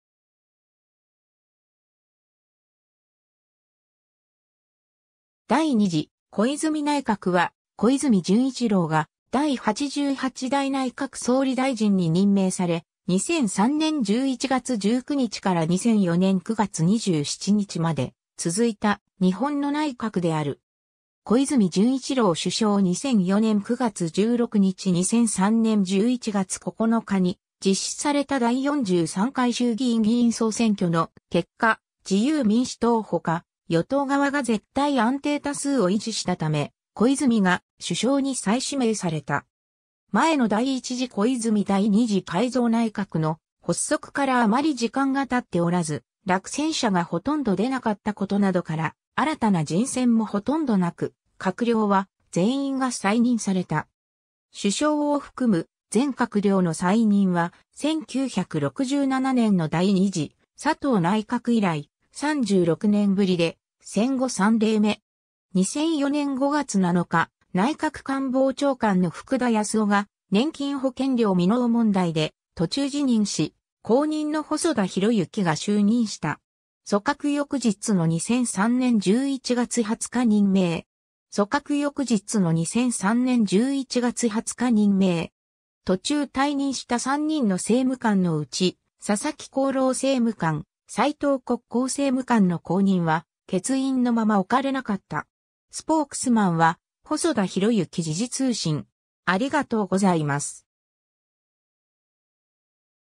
第2次小泉内閣
第2次小泉内閣（だいにじこいずみないかく）は、小泉純一郎が第88代内閣総理大臣に任命され、2003年（平成15年）11月19日から2004年（平成16年）9月27日まで続いた日本の内閣である。
 小泉純一郎首相 2004年（平成16年）9月16日 2003年（平成15年）11月9日に実施された第43回衆議院議員総選挙の結果、自由民主党ほか与党側が絶対安定多数を維持したため、小泉が首相に再指名された。
前の第1次小泉第2次改造内閣の発足からあまり時間が経っておらず、落選者がほとんど出なかったことなどから、新たな人選もほとんどなく、閣僚は全員が再任された。
首相を含む全閣僚の再任は、1967年（昭和42年）の第2次佐藤内閣以来、36年ぶりで、戦後3例目。
 2004年（平成16年）5月7日、内閣官房長官の福田康夫が年金保険料未納問題で途中辞任し、後任の細田博之が就任した。
 組閣翌日の2003年（平成15年）11月20日任命。
 ※ 途中退任した3人の政務官のうち佐々木厚労政務官、斉藤国交政務官の後任は欠員のまま置かれなかった。
 ^ スポークスマンは細田 博之 - 時事通信